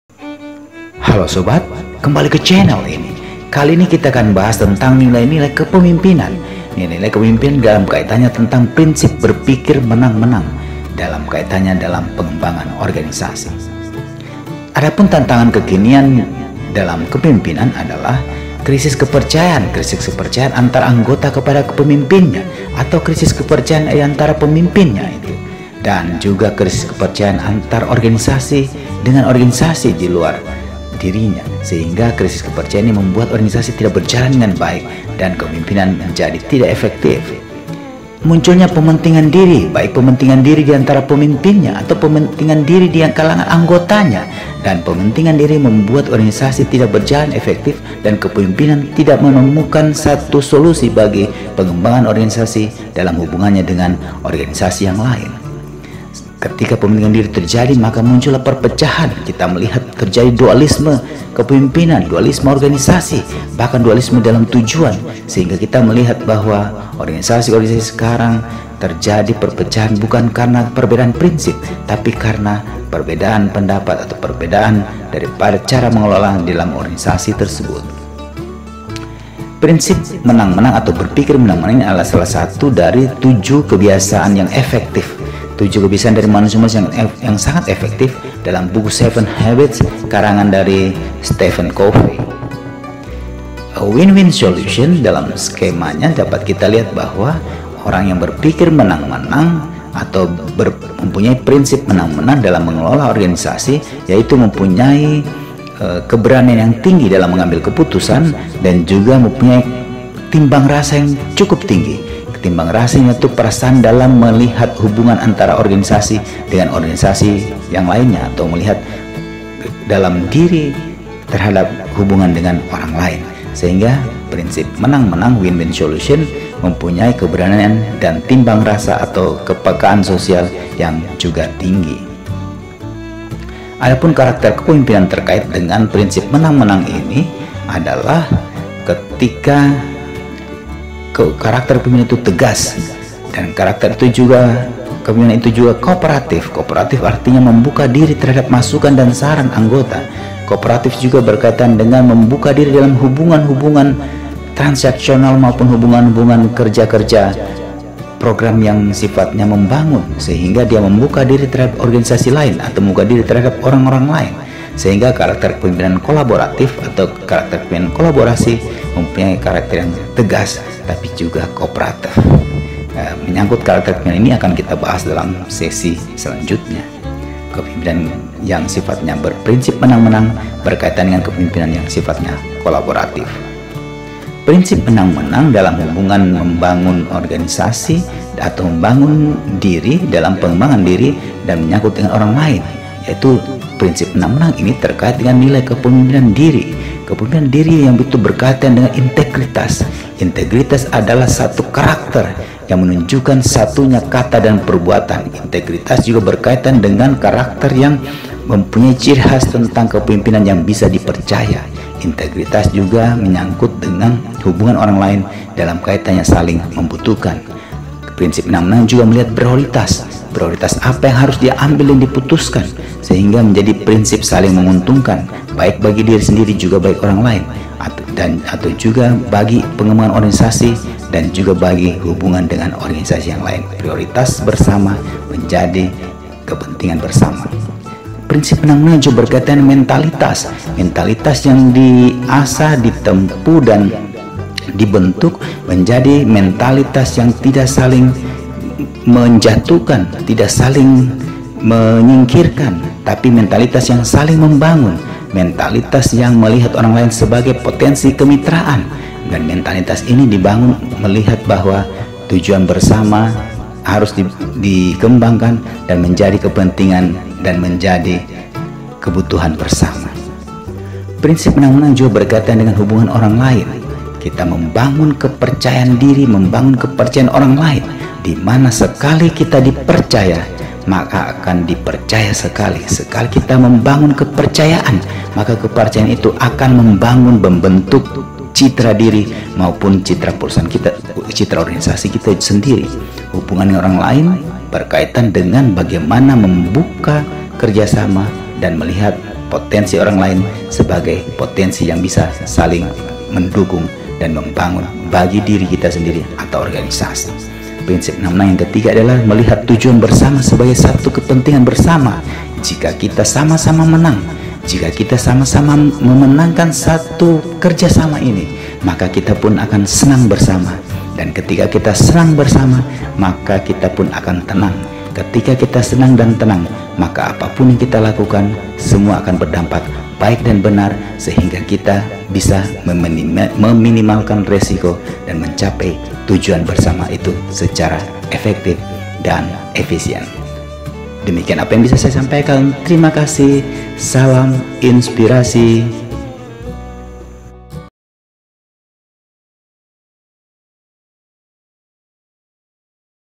WIN-WIN.SOLUTION | Prinsip Berpikir Menang - Menang
Pronsip Berpikir Menang-Menang atau Win - Win Solution merupakan salah satu nilai kepemimpinan baik bagi diri juga dalam manajemen organisasi.